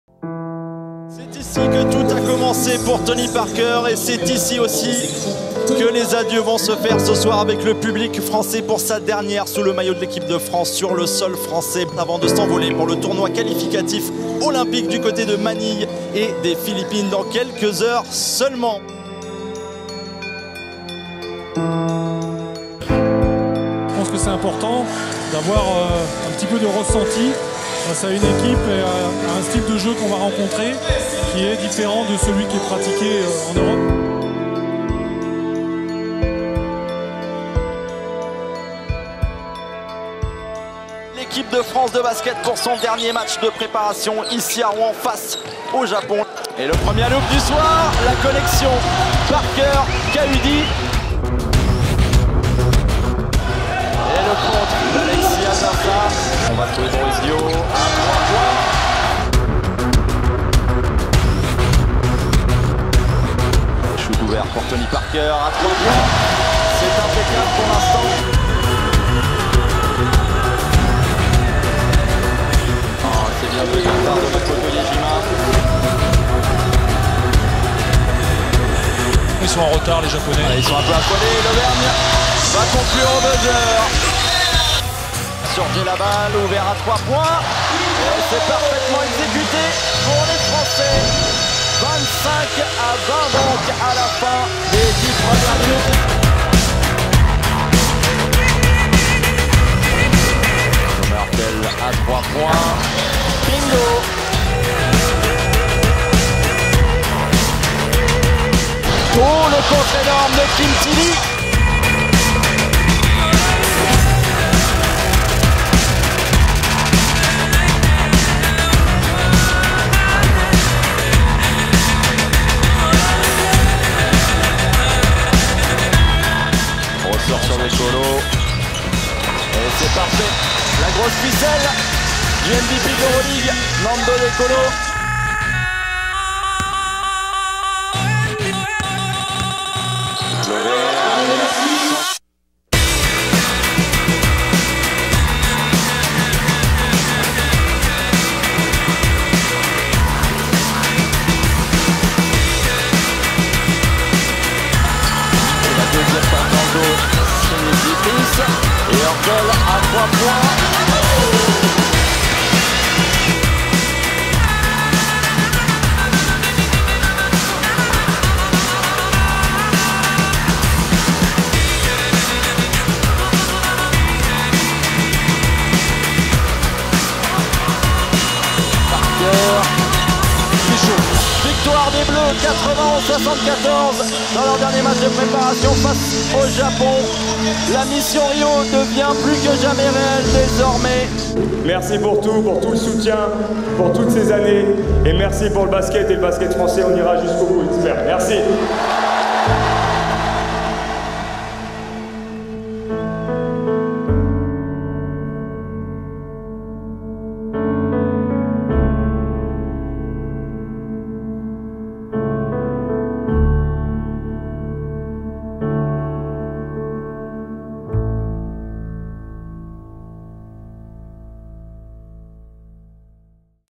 MissionRio - 28 juin - Le mini movie de France-Japon à Rouen
Premier match du TQO le 5 juillet à 15h00 (heure française). Pour vous faire patienter jusqu'à mardi, découvrez le mini-movie du dernier match de Tony Parker, Mickael Gélabale et Florent Piétrus avec le maillot Bleu en France ‪#‎MissionRio‬